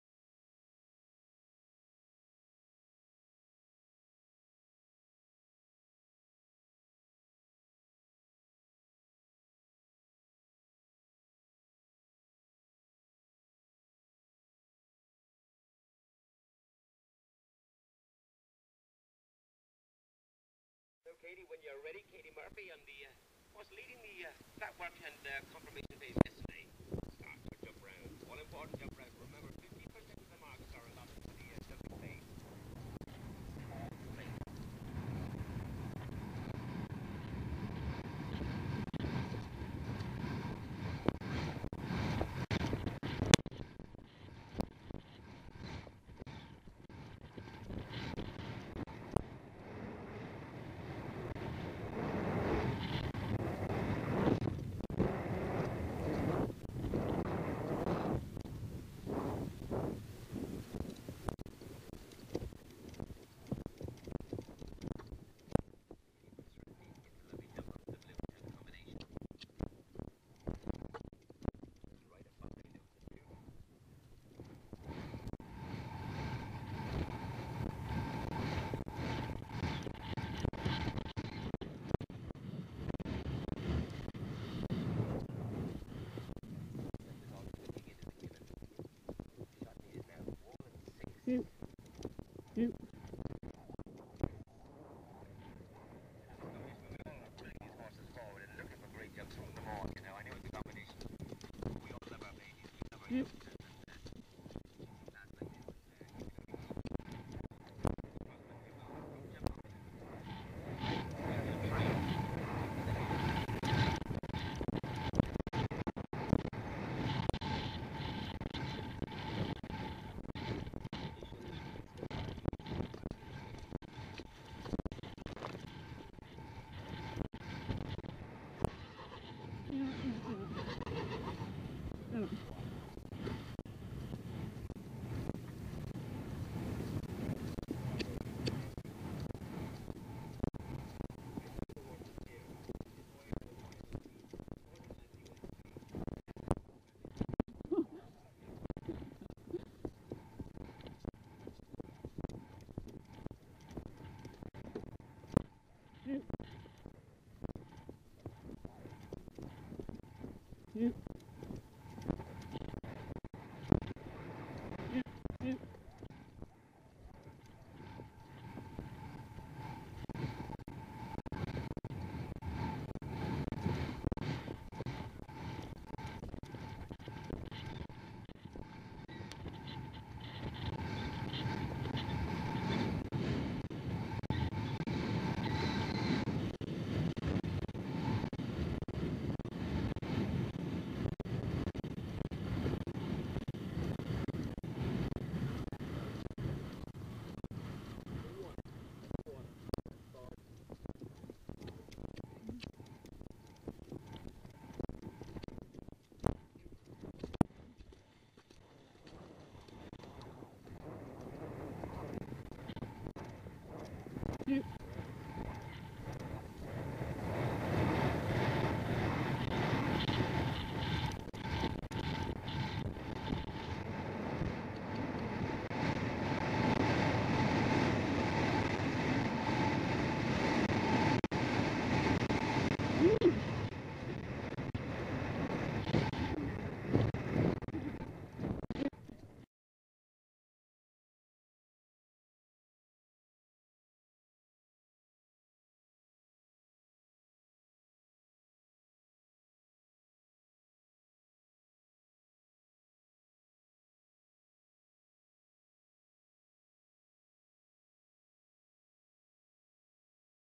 Esccord RGS - Helmet Camerea 4yo YEH East Coast Championships Fair Hill 2012
Esccord RGS
4 year old YEH East Coast Champion

USEA Spalding Labs Young Event Horse East Coast Championships
Fair Hill International
Elkton, Maryland